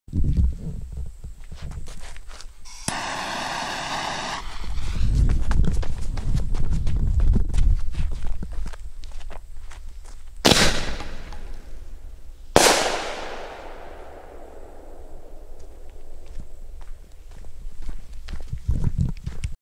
Small Shell Test
Testing out my external microphone, a new batch of lift powder, and Shimizu's green star formula with a hot prime. The shell was a 2.5" paper hemi pasted using the 3 strip method and contained no booster.